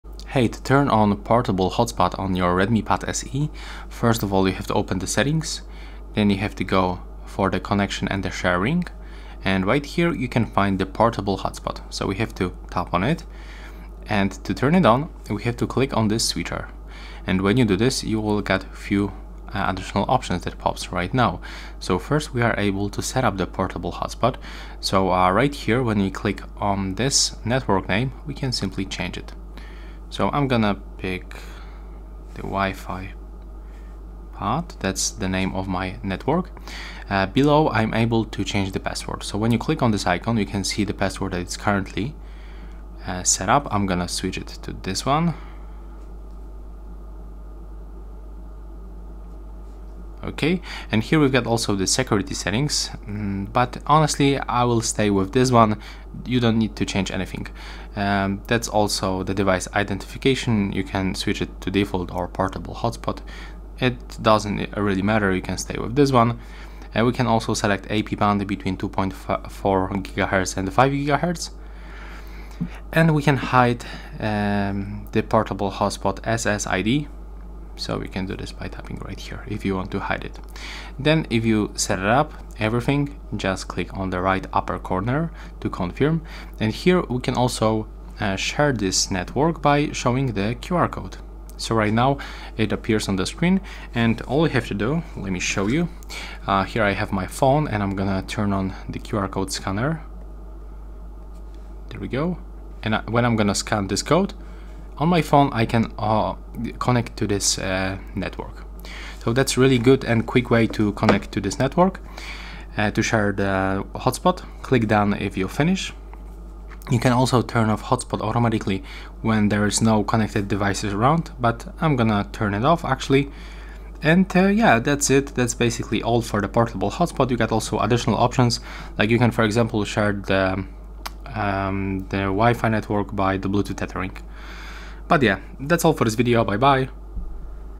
How to Turn On Portable Hotspot on Redmi Pad SE - Share your Internet Connection
I'll walk you through the simple process, allowing you to share your device's internet connection effortlessly with other devices.